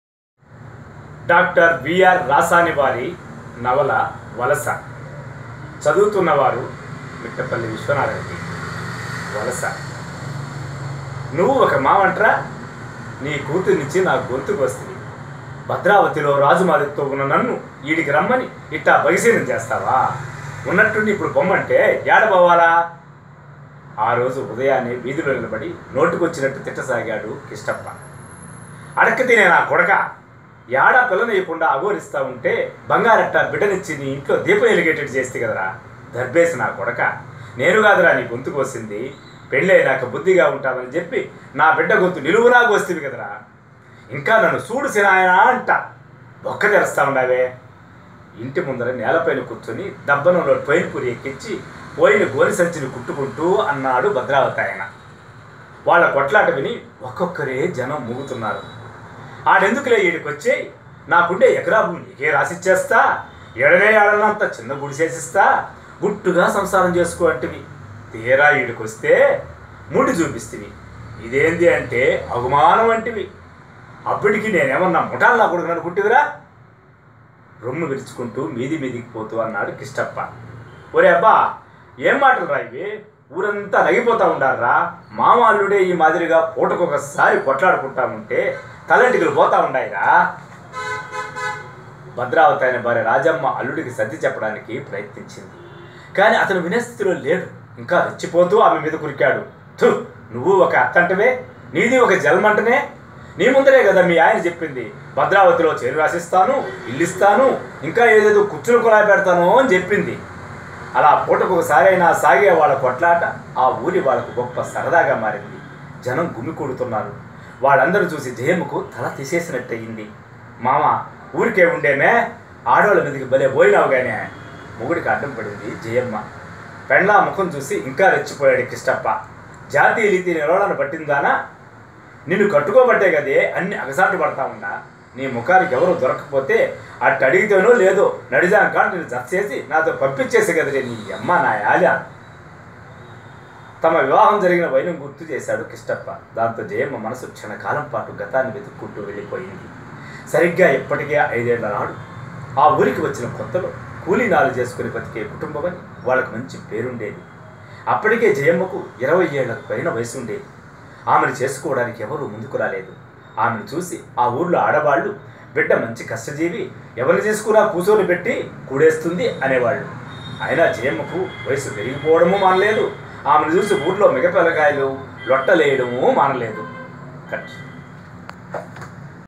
వలస
1960 నుంచి 1990 వరకు నడిచిన తెలుగు పల్లె ముఖ్యంగా చిత్తూరు జిల్లా లోని పల్లె  జీవనాన్ని  కళ్ళకు కట్టిన డా "  రాసానిగారి నవల  ఏదైనా  చదివి తీరాల్సిందే .. దాన్ని చదివితే  ఎలా  ఉంటుందని  చిన్న కుతూహలంతో ...